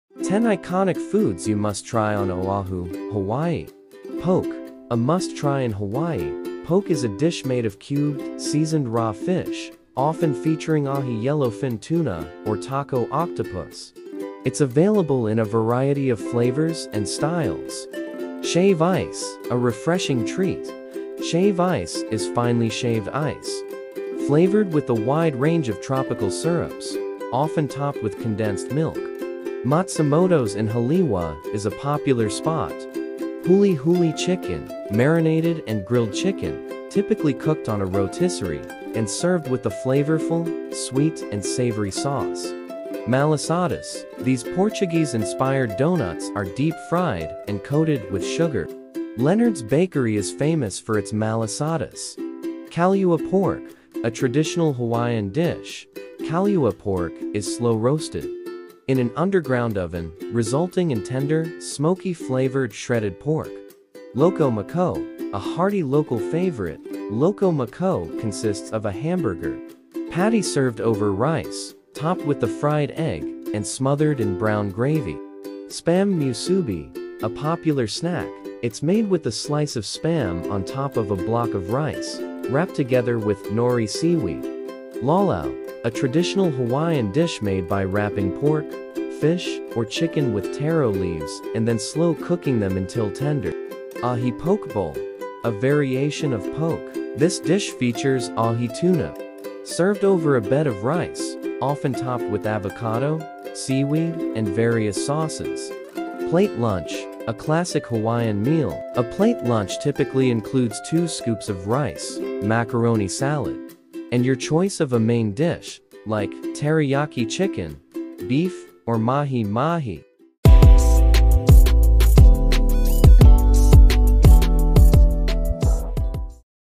The Tastes of Oahu, Hawaii - 10 MUST EATS: Poke, Plate Lunch, Malasadas and More
Oahu, Hawaii, offers a rich and diverse food culture influenced by its unique blend of cultures. When visiting Oahu, make sure to try these 10 iconic foods that showcase the flavors of the island:

Poke: A must-try in Hawaii, poke is a dish made of cubed, seasoned raw fish, often featuring ahi (yellowfin tuna) or tako (octopus). It's available in a variety of flavors and styles.

Shave Ice: A refreshing treat, shave ice is finely shaved ice flavored with a wide range of tropical syrups, often topped with condensed milk. Matsumoto's in Haleiwa is a popular spot.

Huli Huli Chicken: Marinated and grilled chicken, typically cooked on a rotisserie, and served with a flavorful sweet and savory sauce.

Malasadas: These Portuguese-inspired doughnuts are deep-fried and coated with sugar. Leonard's Bakery is famous for its malasadas.

Kalua Pork: A traditional Hawaiian dish, kalua pork is slow-roasted in an underground oven, resulting in tender, smoky-flavored shredded pork.

Loco Moco: A hearty local favorite, loco moco consists of a hamburger patty served over rice, topped with a fried egg and smothered in brown gravy.

Spam Musubi: A popular snack, it's made with a slice of Spam on top of a block of rice, wrapped together with nori (seaweed).

Laulau: A traditional Hawaiian dish made by wrapping pork, fish, or chicken with taro leaves and then slow-cooking them until tender.

Ahi Poke Bowl: A variation of poke, this dish features ahi tuna served over a bed of rice, often topped with avocado, seaweed, and various sauces.

Plate Lunch: A classic Hawaiian meal, a plate lunch typically includes two scoops of rice, macaroni salad, and your choice of a main dish, like teriyaki chicken, beef, or mahi-mahi.

While these are some iconic foods to try in Oahu, the island offers a wide range of culinary delights, including food truck specialties and fusion cuisine that blends Hawaiian, Asian, and American flavors. Exploring local markets and trying regional dishes is a great way to experience the island's diverse and delicious food culture.

0:00 Poke
0:18 Shave Ice
0:33 Huli Huli Chicken
0:43 Malasadas
0:53 Kalua Pork
1:04 Loco Moco
1:15 Spam Musubi
1:25 Laulau
1:35 Ahi Poke Bowl
1:46 Plate Lunch
2:00 End